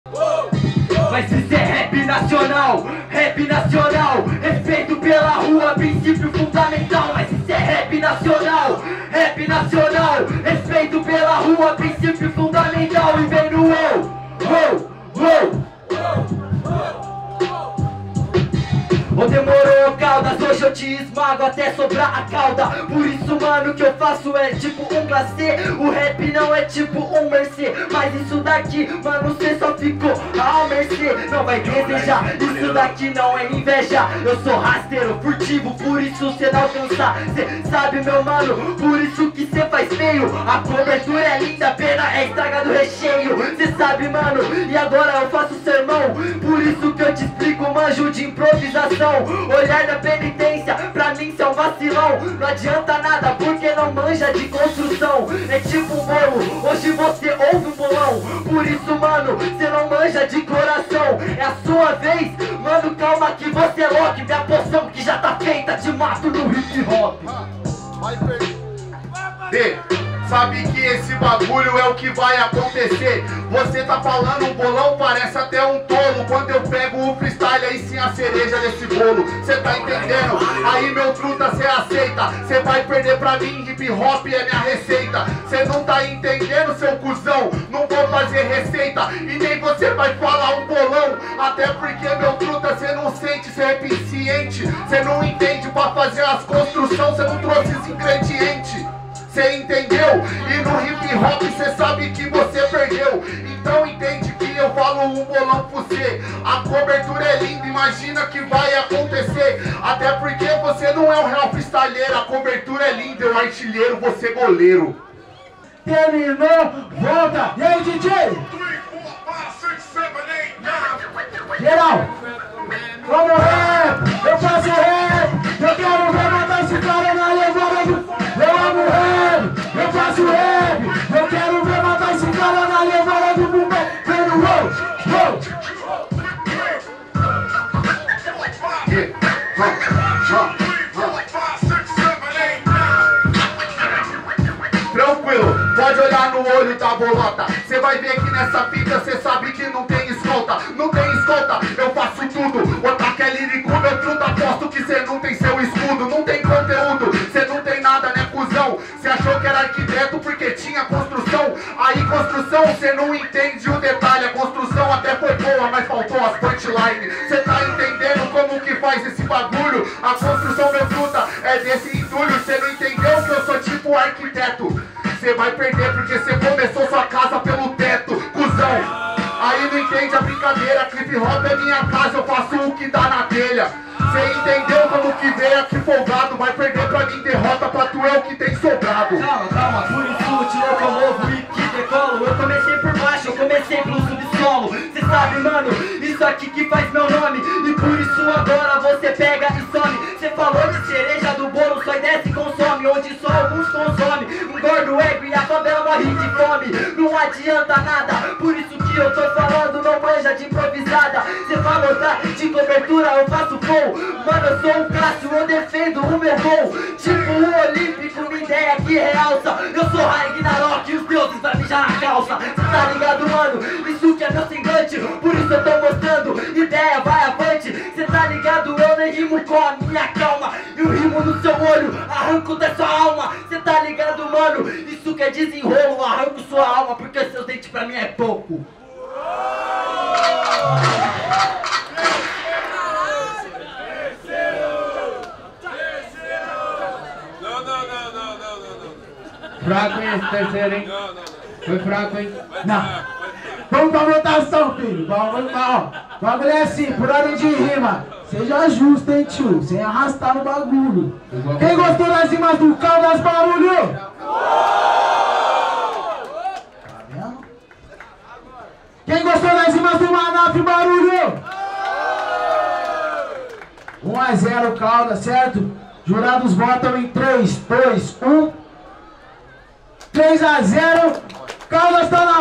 Manaff x Caldas | Segunda fase | CENTRAL 14 ANOS - Junho 2023
A Batalha Central ocorre na Praça José Bonifácio em Piracicaba - SP, toda segunda sexta do mês. Cola com noiz.

Desde 2009 🔥